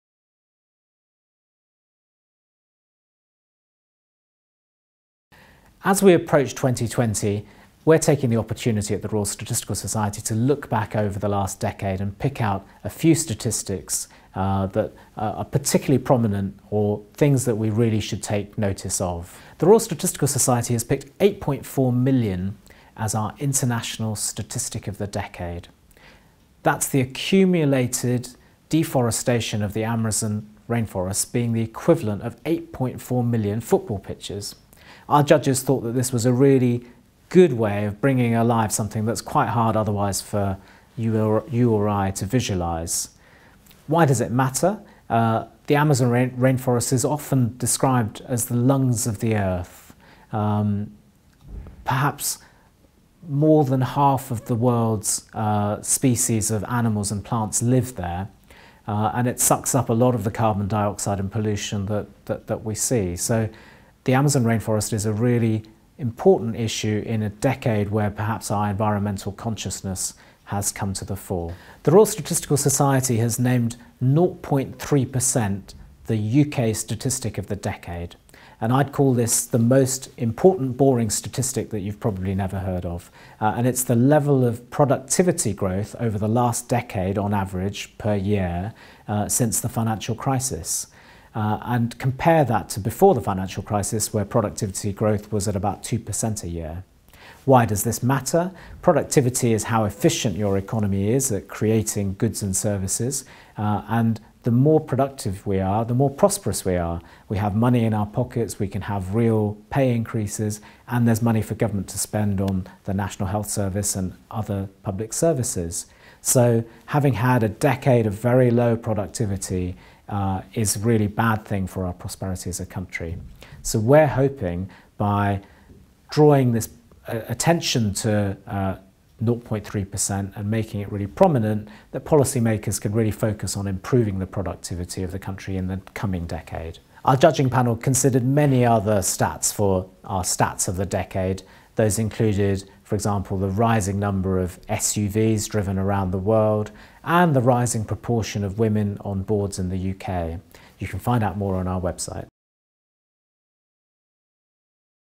Statistics of the Decade 2010-2019
Hetan Shah, Executive Director of the Royal Statistical Society (RSS) announces the Statistics of the Decade, 2010 – 2019.